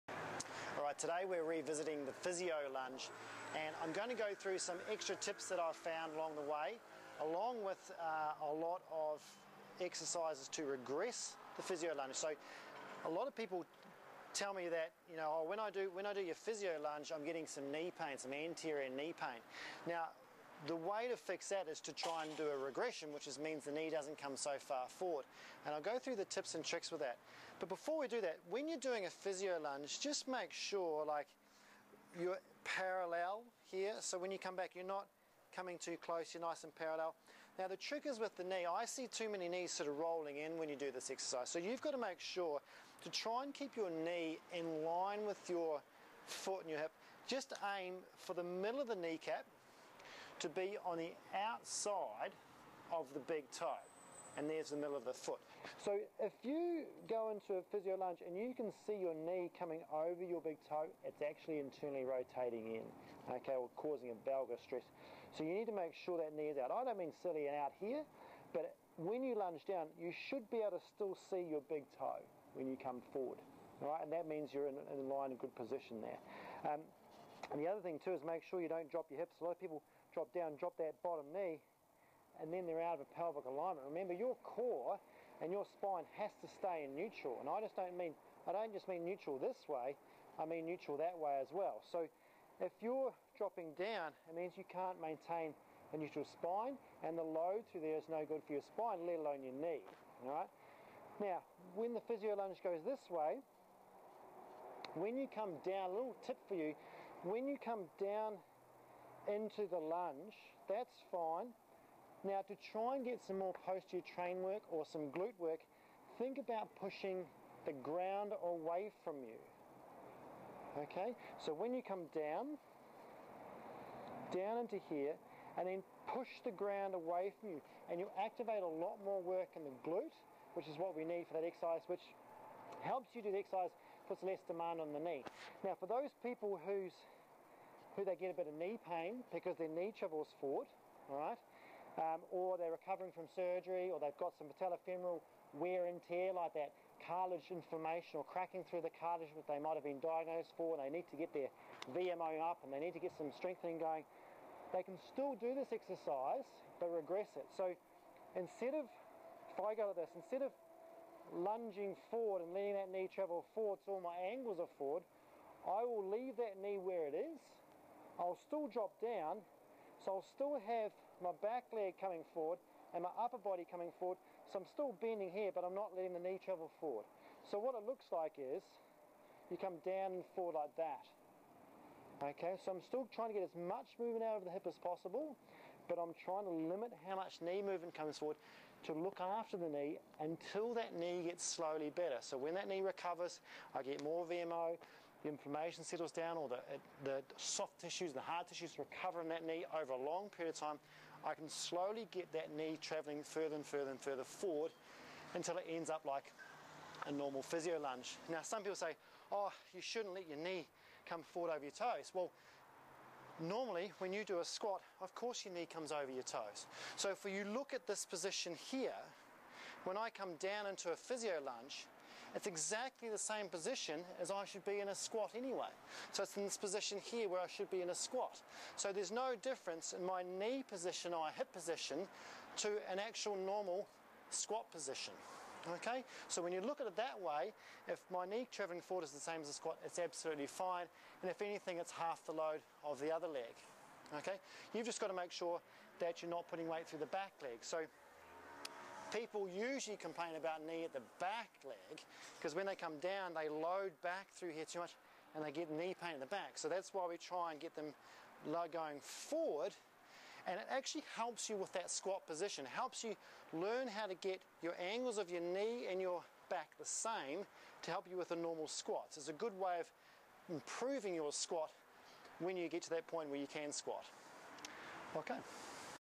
The Physio Lunge - Pt 2: Patello-femoral pain and angles| Feat. Tim Keeley | No.62 | Physio REHAB
The Physio Lunge exercise is re-visited with tips on how to changes the angles of the hip and knee for people with anterior or patello-femoral knee pain, as well as correct knee over toe alignment